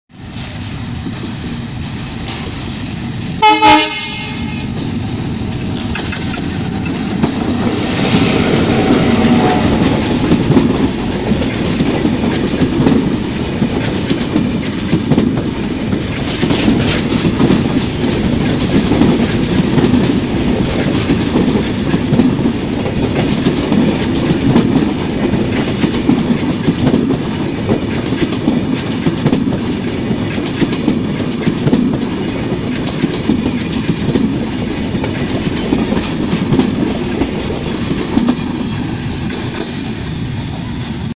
Cleaned from the washpit Khulna ।। Benapole express fast thorough passing Jashore at BD railway
Train code: 795/796
Name of train :  Benapole express
Route : Benapole to Dhaka
Off Day : Wednesday
Type : Intercity
ভাড়াঃ এসি সিট  ১১১৬/-,এসি চেয়ার -৯৩২/-, শোভন চেয়ার ৪৮৫/-
 ( বেনাপোল হইতে ঢাকা)।
# Bangladesh railway
# BD Railway
# Train & Travel Fan